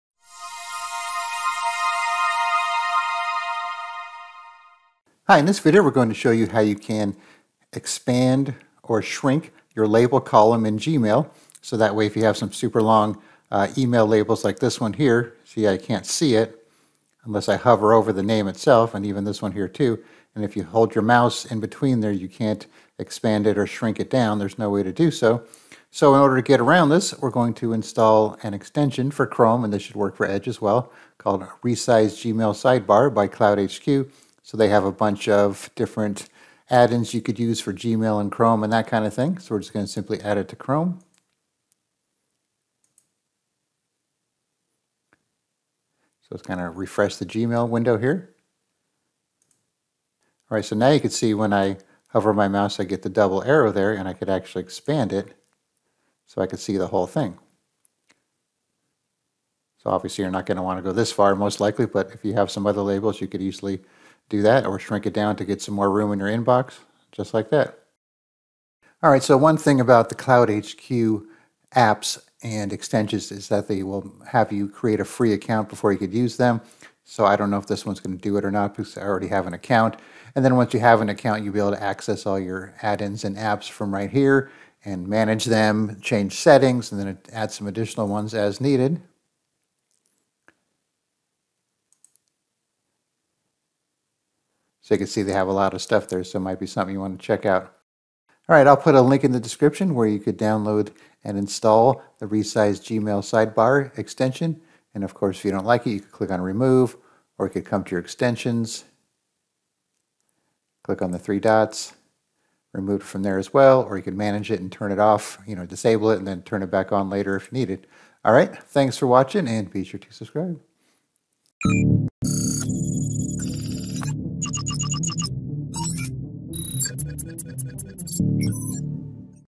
How to Resize Your Gmail Label Sidebar
If you have some extra long label names in your Gmail sidebar, then you might want to try out the free Resize Gmail Sidebar by CloudHQ extension that will let you expand and shrink the size of your Gmail side panel where you find your label names. It's very easy to install and works great.

Here is a link where you can download the Resize Gmail Sidebar extension.

Check out our book titled Gmail Made Easy to take your email management skills to the next level!